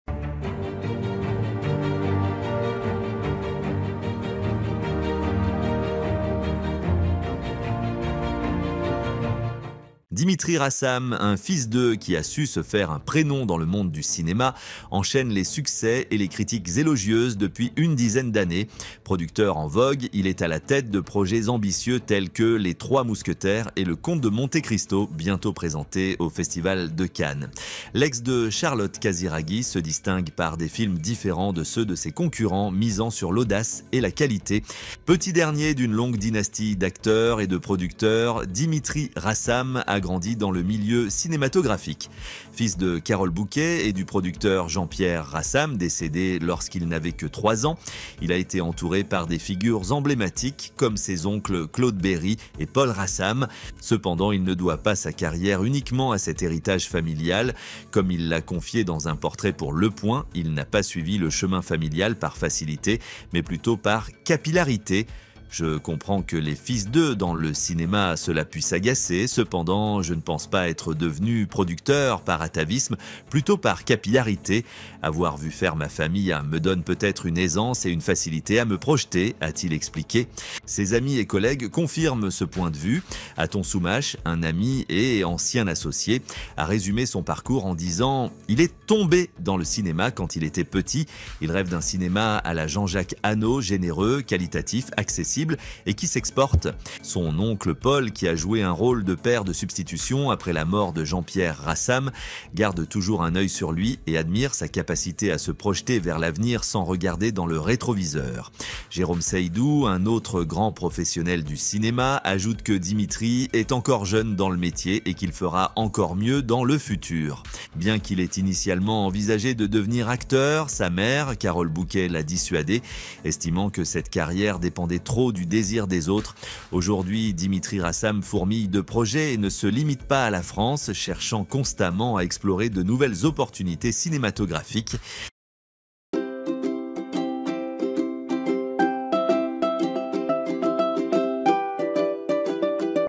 Dimitri Rassam, fils de Carole Bouquet et d'un puissant producteur, assume et répond cash
Dimitri Rassam, un "fils de" qui a su se faire un prénom dans le monde du cinéma, enchaîne les succès et les critiques élogieuses depuis une dizaine d'années. Producteur en vogue, il est à la tête de projets ambitieux tels que Les Trois Mousquetaires et Le Comte de Monte-Cristo, bientôt présenté au Festival de Cannes. L'ex de Charlotte Casiraghi se distingue par des films différents de ceux de ses concurrents, misant sur l'audace et la qualité.